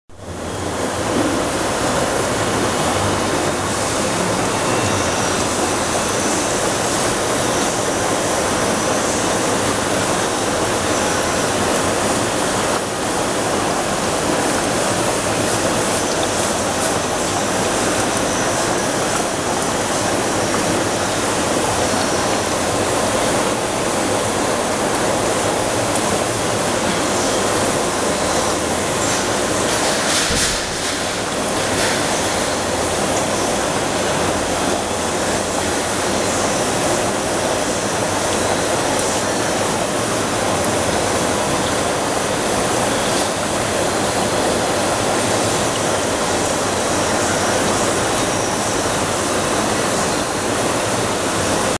Poecilia reticulata male yellow / Gelbes Guppy-Männchen
Gelbe Guppy-Männchen in der Größe von 3 bis 4 cm.